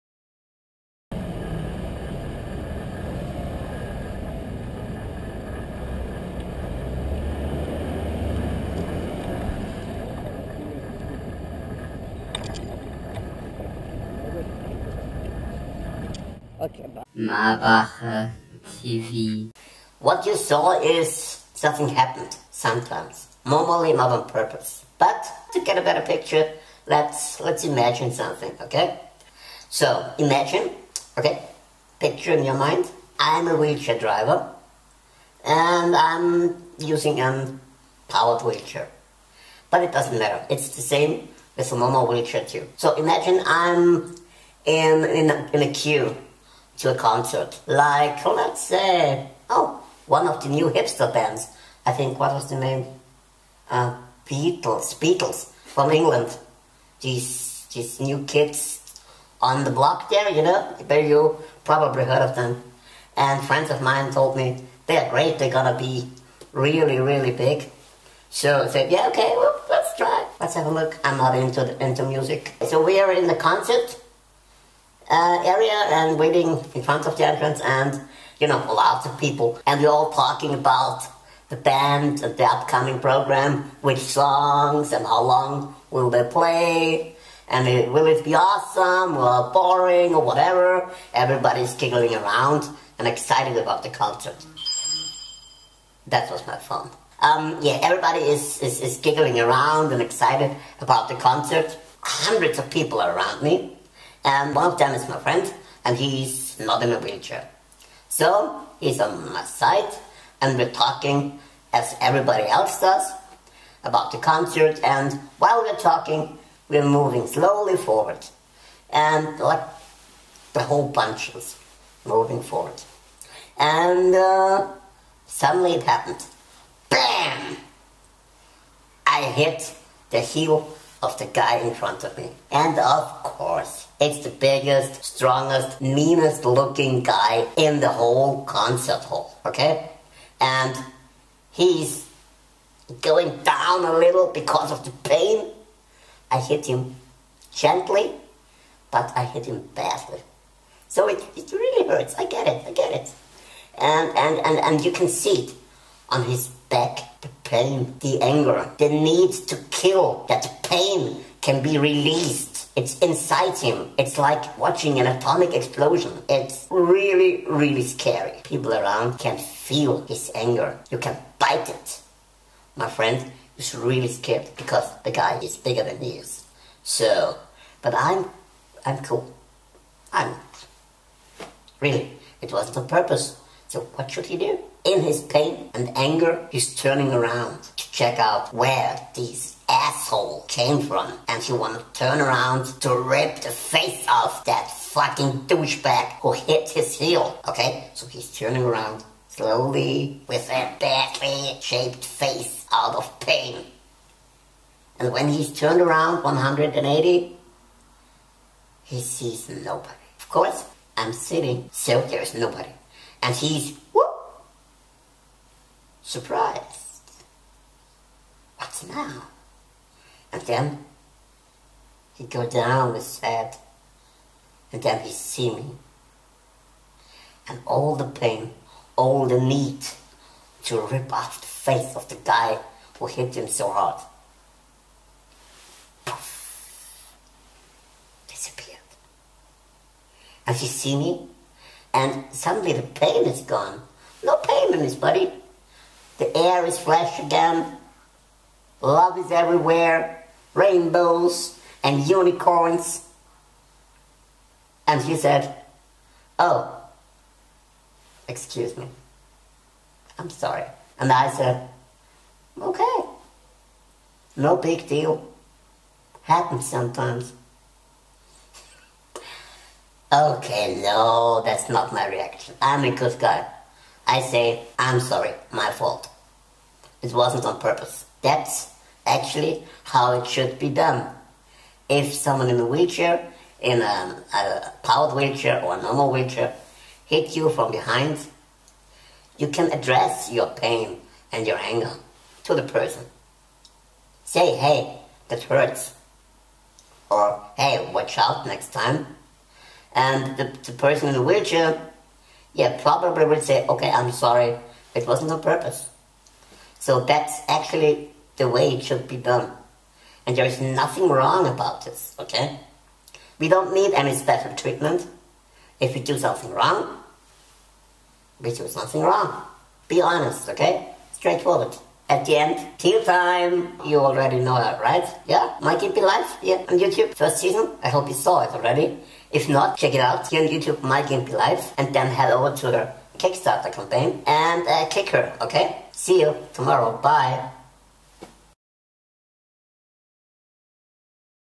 MGL - 5 Days! Don't apologize
- to make sure Teal is getting ready for a second season of MGL!

So kick her on Kickstarter.com — and become a Backer!

I will release every day one video with one (hopefully) usefull advice to 'how do i talk to people in a wheelchairs?'. At the end of each video i will reminde you to „kick" Teals defenetly perfect hidden, but with absolutely certainty cute ass on kickstarter.com! ;-)

The campaign will end on June 14th.

Looking forward to MGL season 2!!!